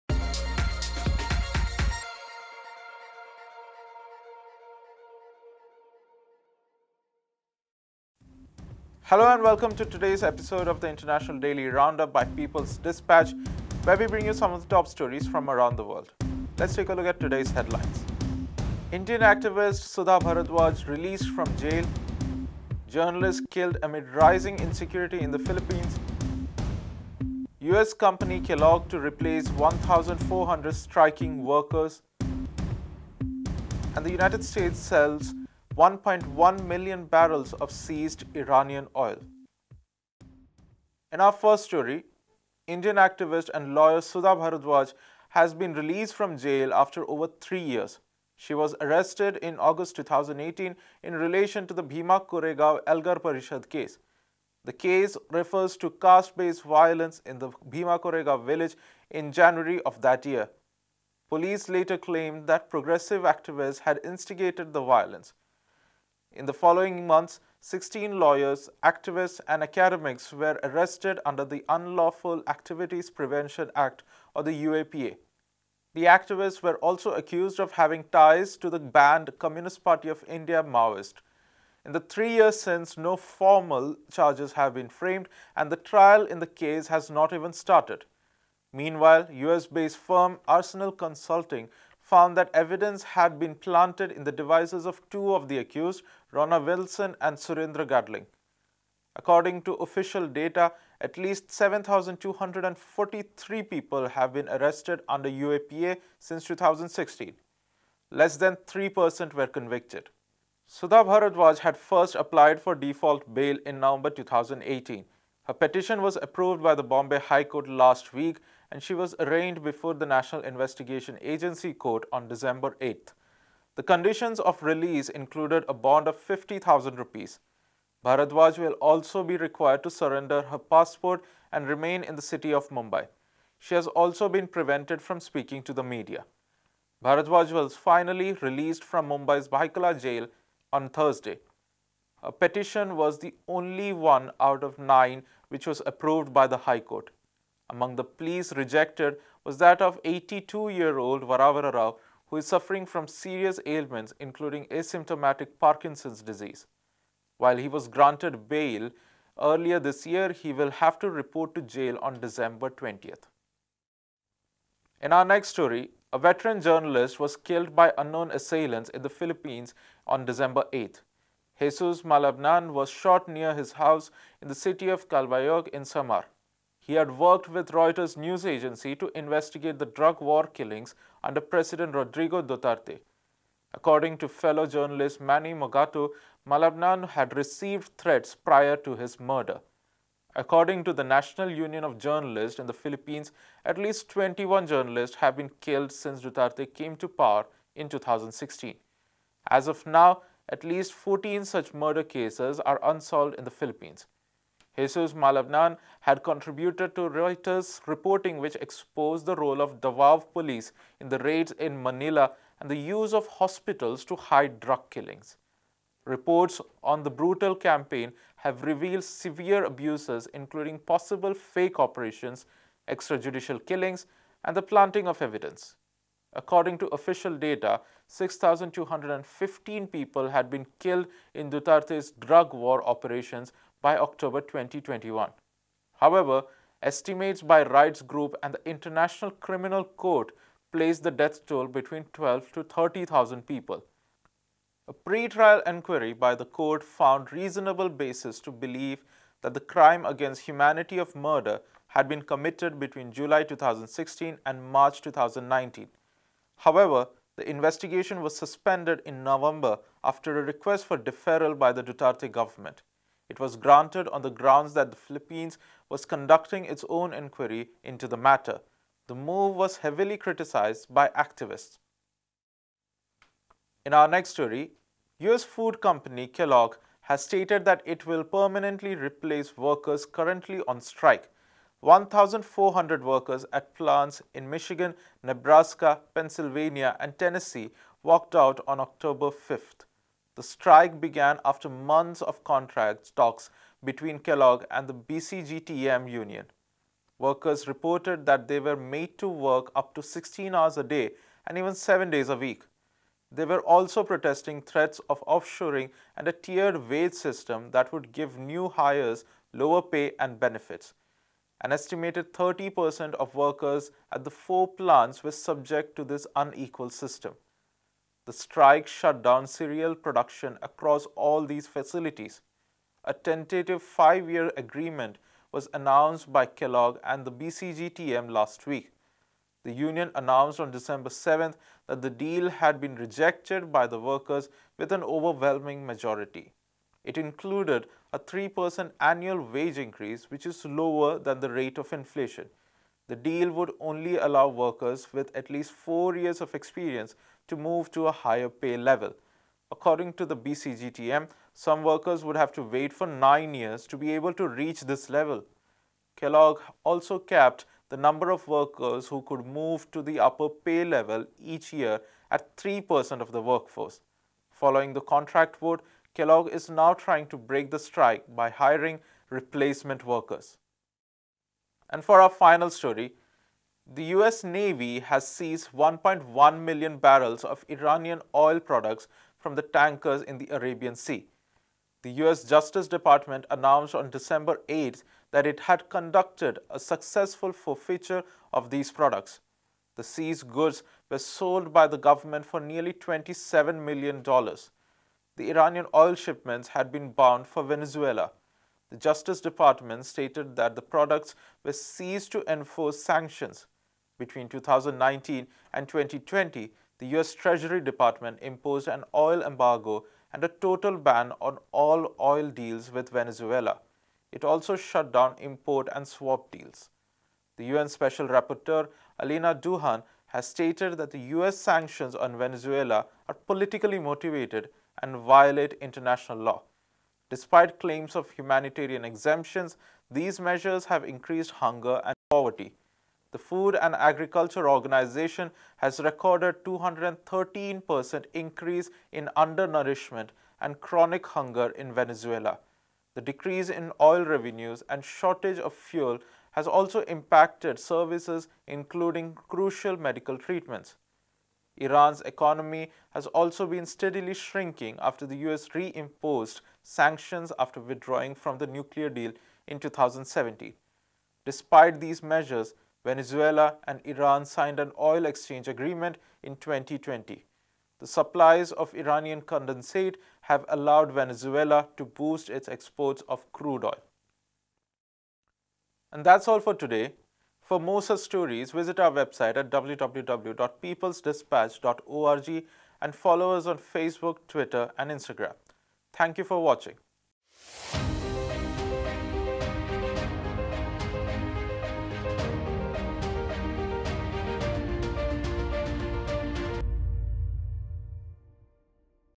Daily Round-up|Sudha Bharadwaj granted bail in Bhima Koregaon case after 3 years and other stories
In today’s episode of the Daily Round-up we look at the default bail granted to Indian activist and lawyer Sudha Bharadwaj who was among 16 people detained in relation to the Bhima Koregaon Elgar Parishad case; the murder of journalist Jesus “Jess” Malabanan in the Philippines and rising insecurity under President Duterte; the ongoing strike by 1,400 workers across Kellogg plants in the U.S. and the company’s decision to permanently replace the striking workers; and the capture and sale of 1.1 million barrels of Iranian oil products fron tankers headed to Venezuela by the U.S. Navy.